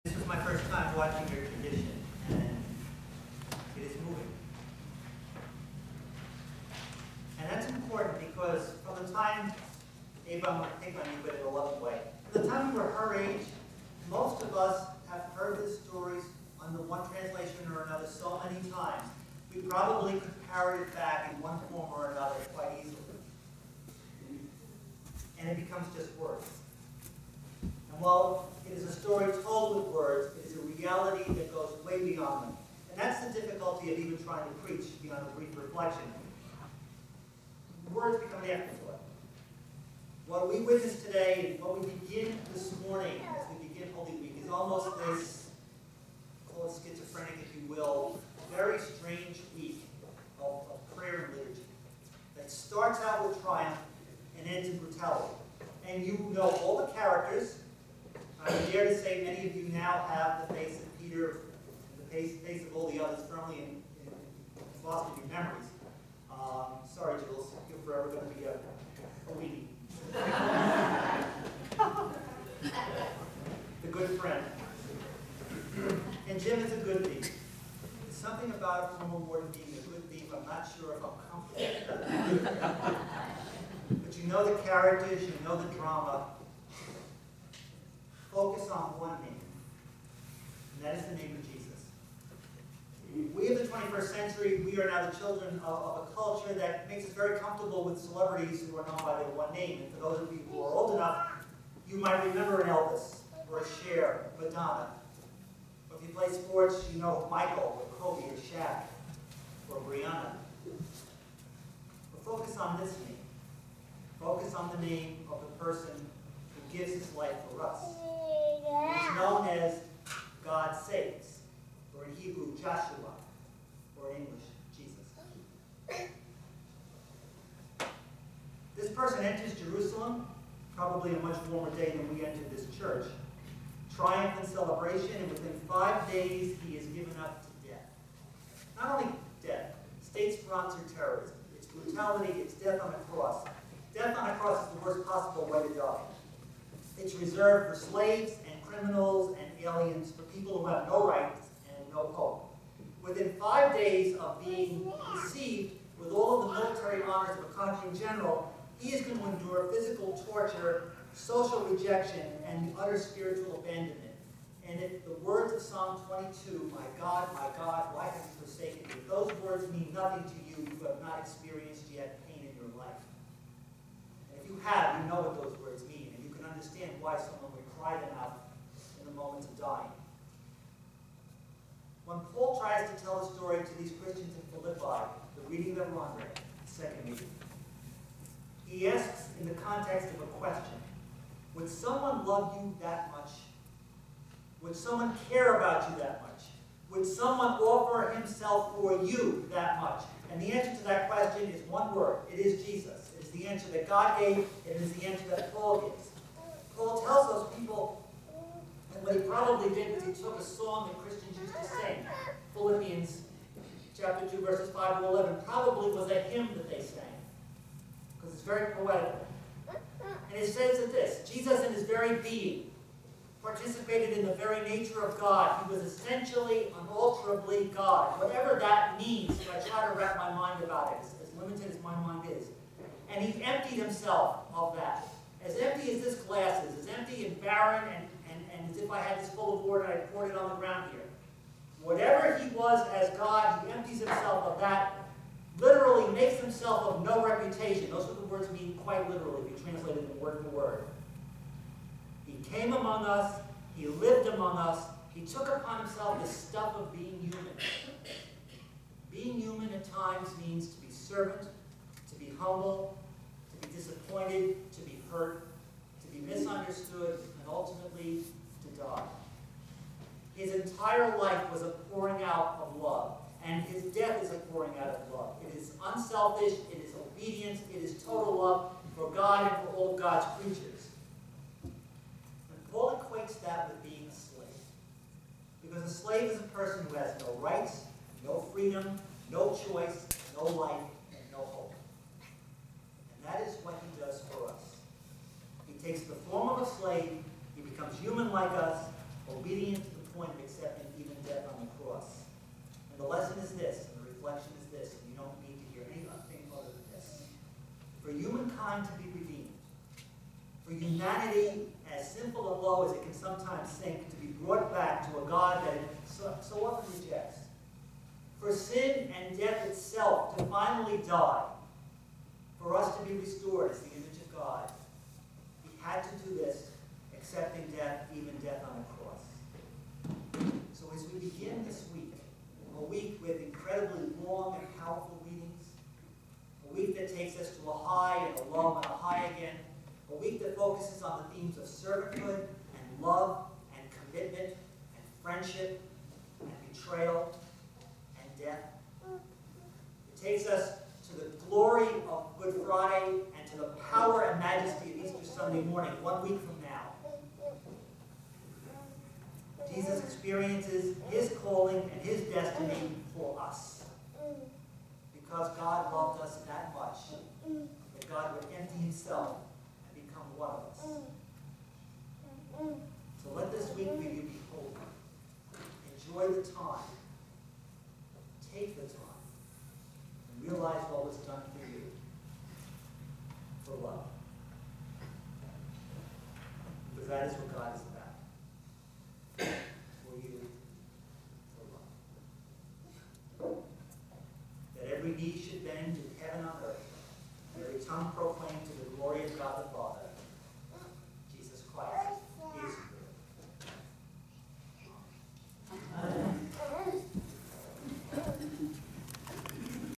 Palm Sunday - March 24, 2013 (Reflection)
This video is of the reflection on the Passion delivered by Fr. Joe Campo during the 9:30 AM liturgy at St. John's